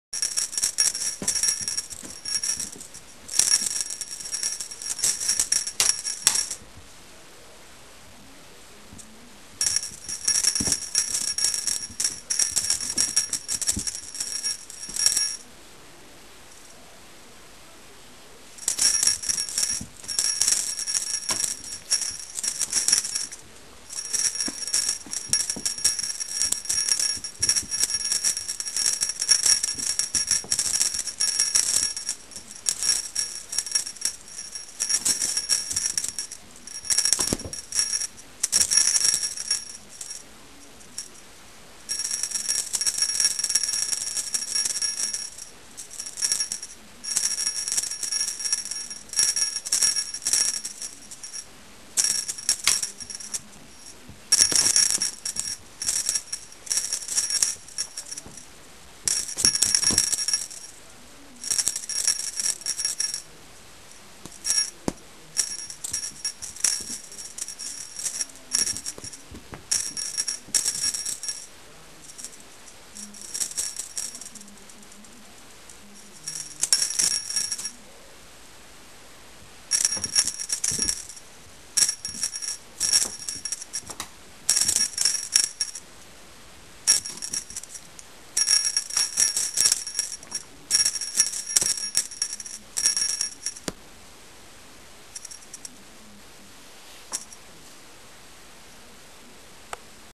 Mystearica's Cat Toy
My new kitten, Mystearica (Tear for short), playing with her new cat toy.